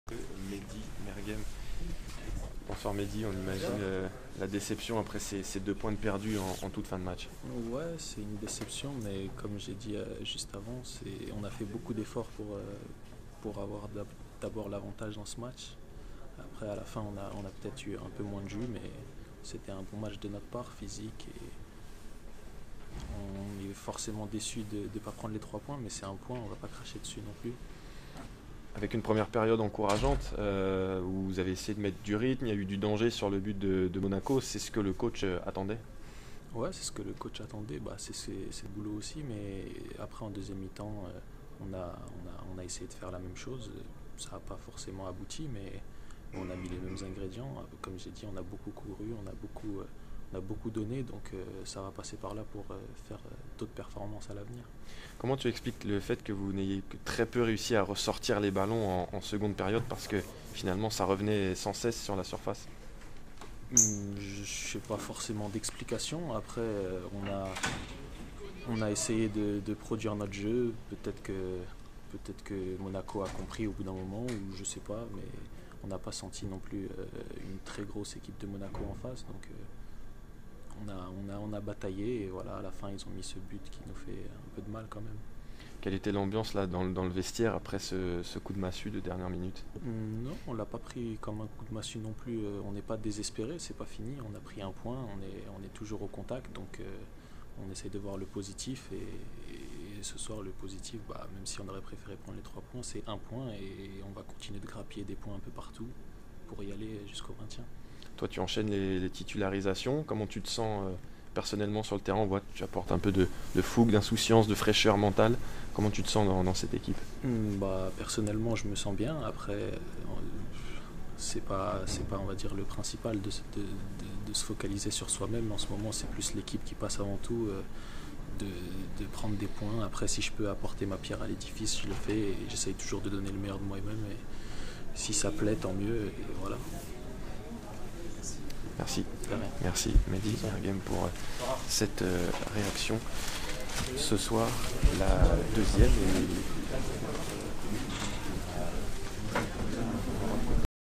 MEHDI MERGHEM RÉAGIT APRÈS GUINGAMP - MONACO (1-1) / Ligue 1 - 6 avril 2019
L'En Avant de Guingamp a été rejoint au score dans les dernières secondes du temps additionnel. Stevan Jovetic a égalisé, profitant d'un cafouillage dans la surface. Il répond à Félix Eboa Eboa, auteur d'une tête à la 23e minute.

Le partage des points est logique dans cette rencontre de la 31e de journée de Ligue 1 de football, où chaque équipe a gagné une période. Il n'en est pas moins rageant pour Guingamp, dernier de Ligue 1, malgré la défaite caennaise face à Nîmes.

Merci pour vos encouragement à coup de pouces bleus et de commentaires, qui améliorent aussi le référencement ;) 👍

STATISTIQUES

37 Possessions (%) 63
11 Tirs 18
2 Tirs cadrés 5
60 Passes réussies (%) 80
2 Hors-jeux 1
3 Corners 9
14 Centres 37
17 Fautes 14
4 Cartons jaunes 5
0 Cartons rouges 0

LES AUTRES RÉACTIONS

Bientôt...

REJOIGNEZ-MOI !